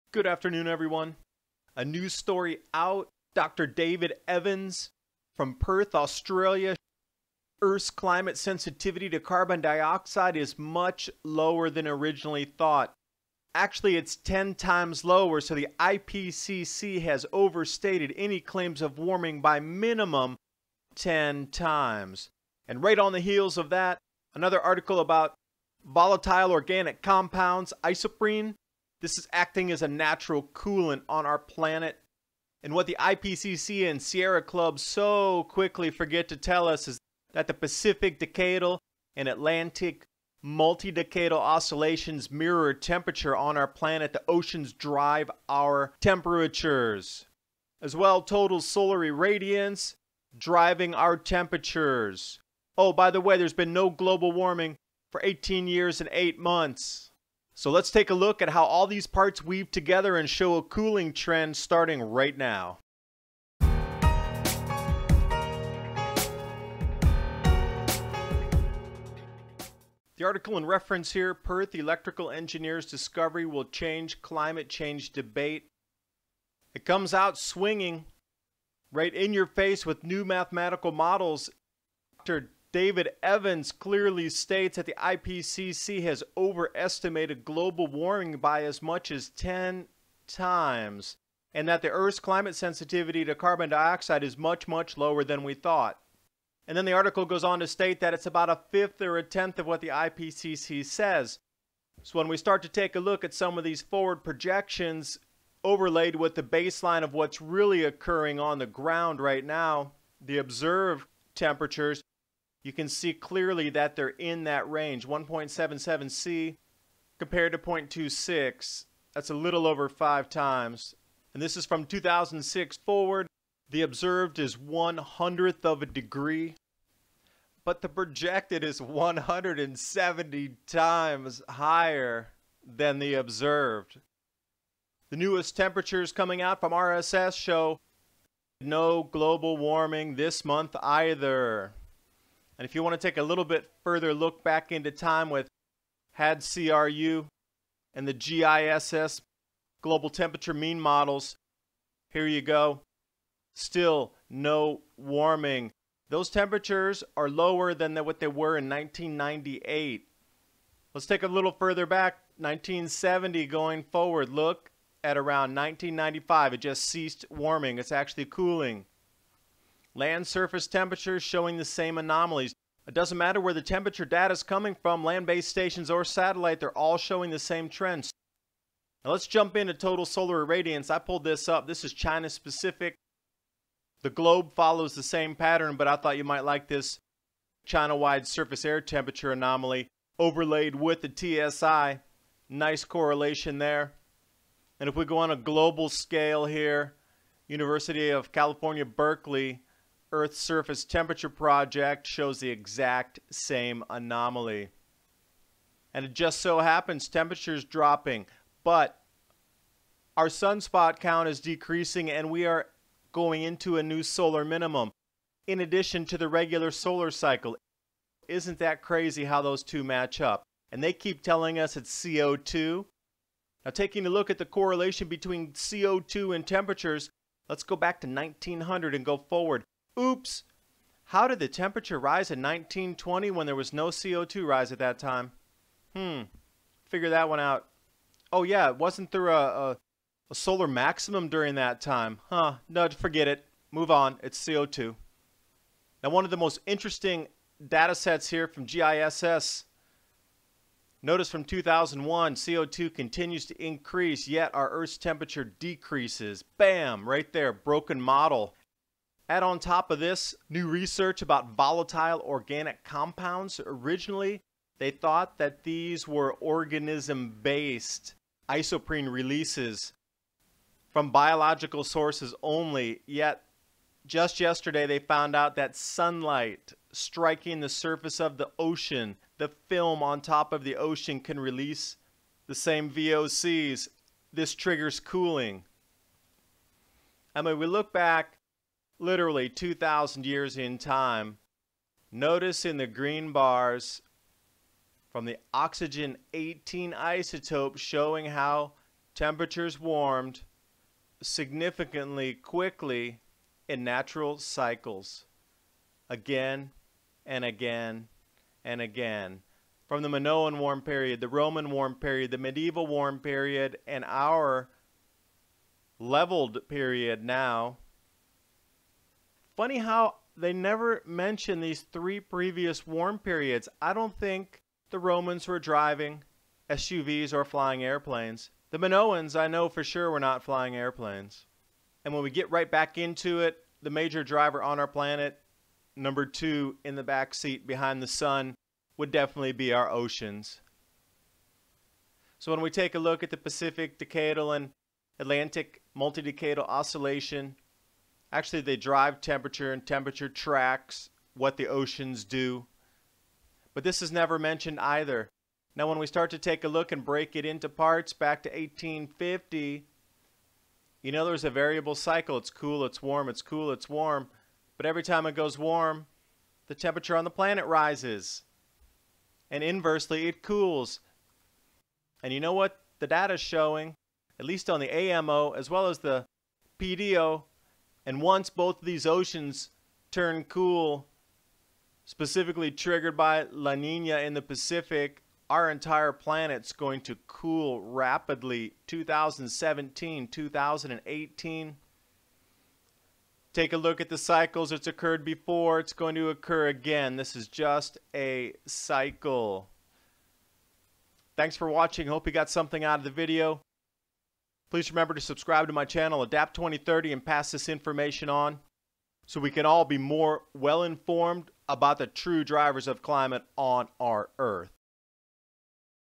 IPCC’s Warming Models are OFF by a Staggering 10X | Mini Ice Age 2015-2035 (96)
New models by Perth-based electrical engineer Dr. David Evans show that the IPCC’s warming models are off by a staggering 10X, a 1000% to be dramatic!